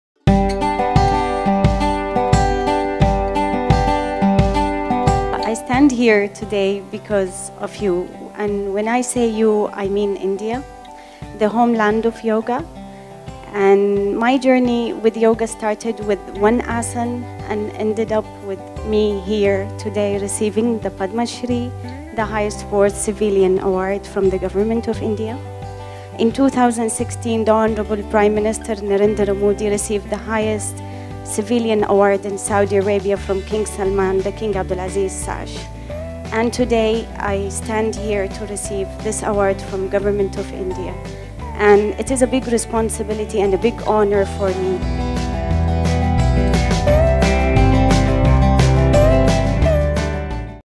Nouf Marwaai speaks at an Interaction with Padma Awardees 2018
Nouf Marwaai shares her thoughts on receiving Padma award 2018 at an interaction hosted by Ministry of Home Affairs.